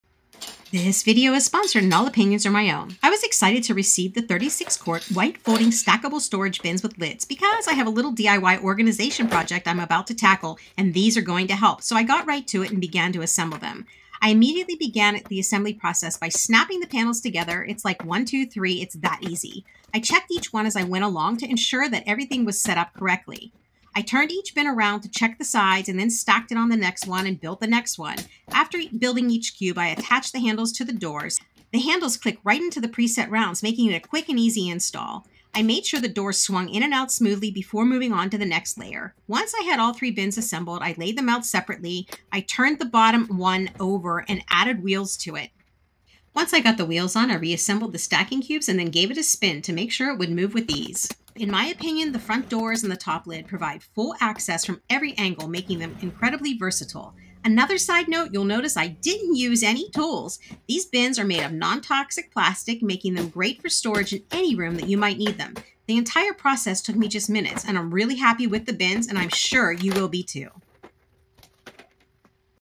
Stackable Foldable Organizational Cubes You've GOt To See How They Work!
If you are looking to organize a space check out the foldable stackable mini cabinets that will help you bring chaos to order. Organizing in minutes after you set these cubes up!